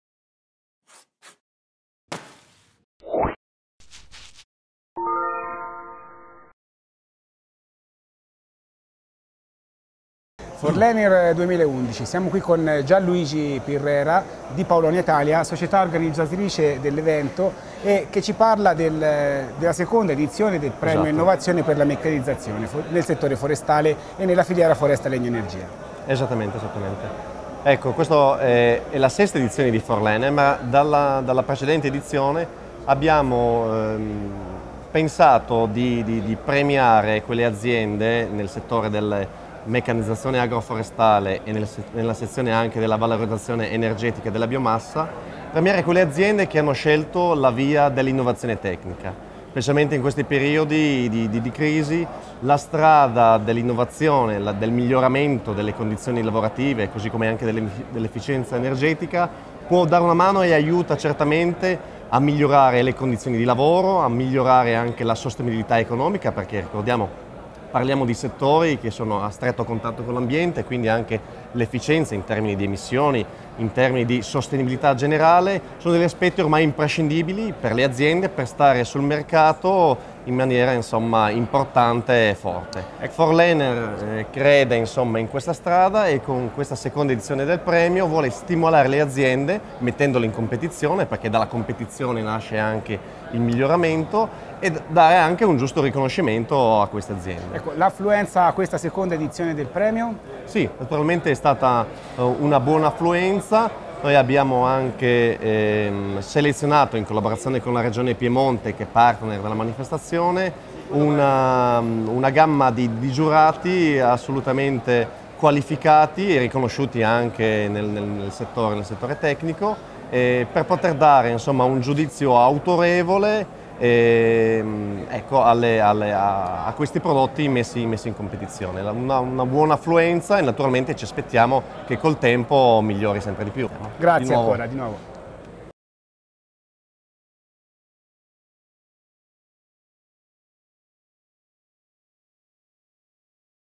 Premio Innovazione Tecnica Forlener 2011
Gianluigi Pirrera, di Paulownia Italia, presenta la seconda edizione del  "Premio Innovazione Forlener 2011" alla quale hanno partecipato ditte che producono e/o commerciano macchine e attrezzature forestali, iscritte alla "Sezione Utilizzazione e Meccanizzazione forestale", e impianti termici a biomassa legnosa ed energie rinnovabili, iscritte alla "Sezione Termotecnica". I presidenti di giuria delle due sezioni sono rispettivamente il Dr. Raffaele Spinelli del CNR-IVALSA e l'Ing. Roberta Roberto di ENEA.